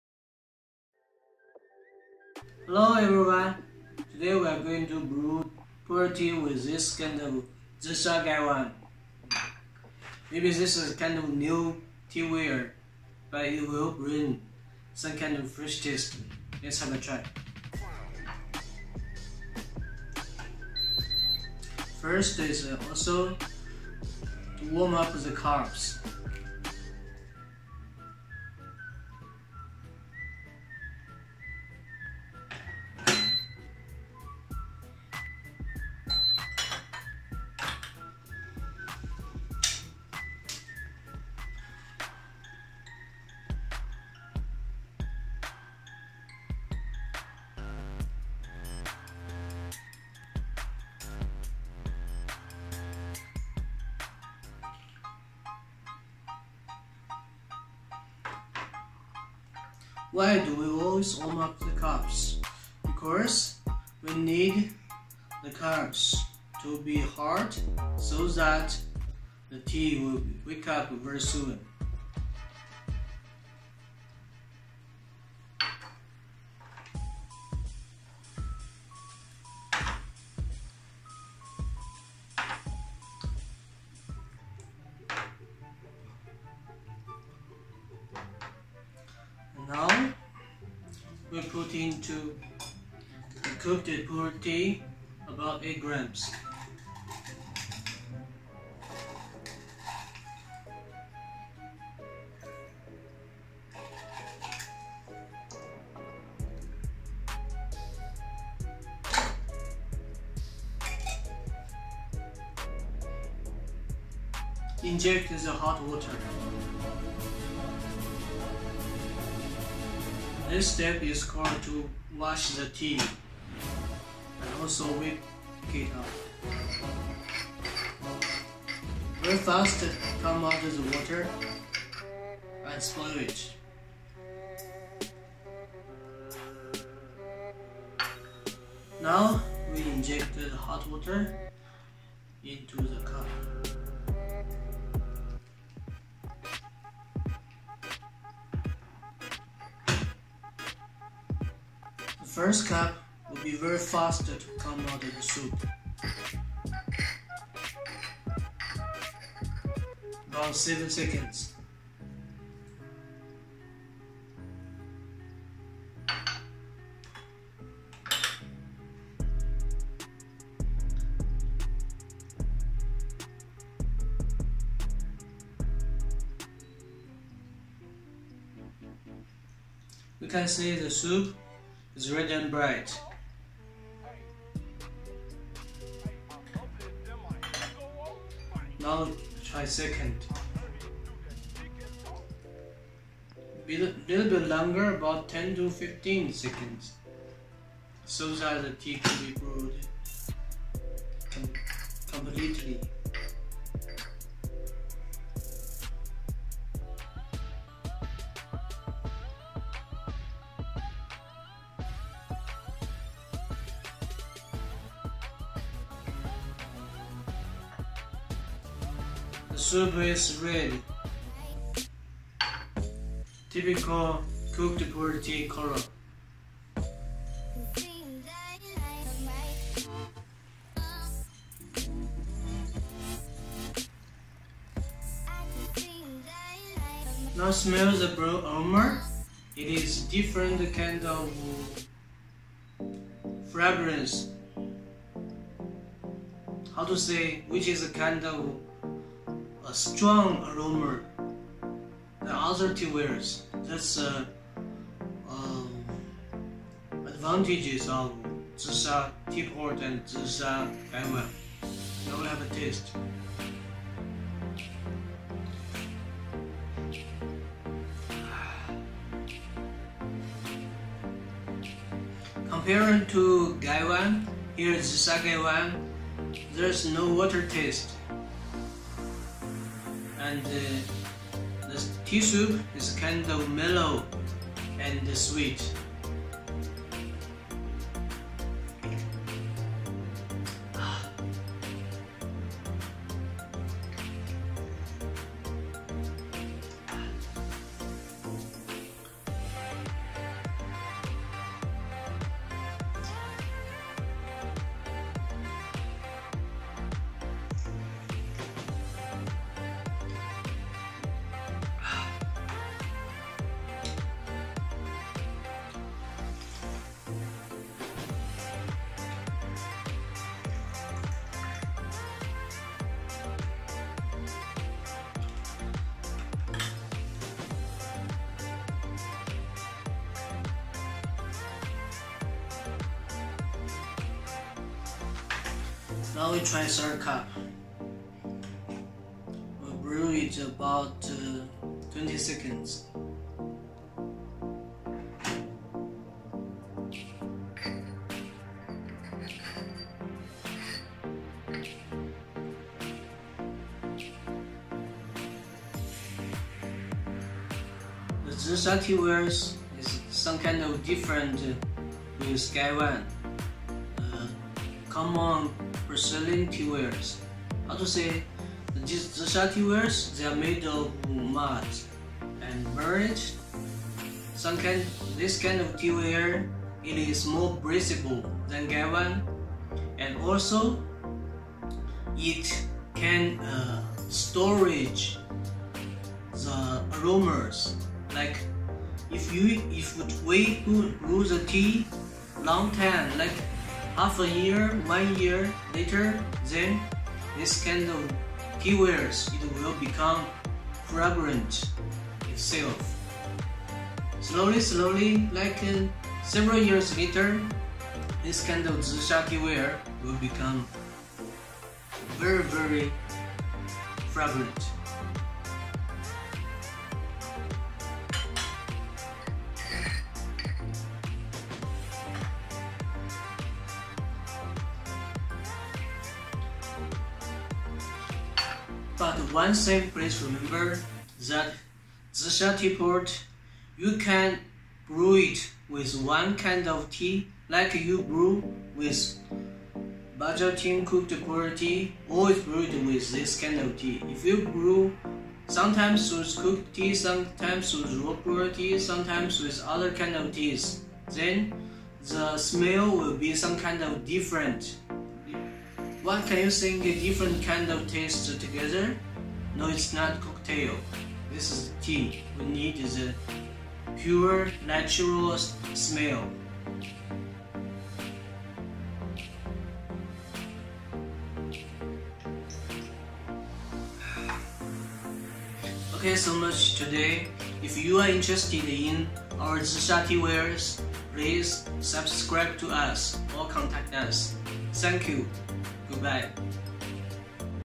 How to Brew Pu erh with Zisha Gaiwan
Zisha teapot and Gaiwan are most common teawares. Today let's take a look at how it comes!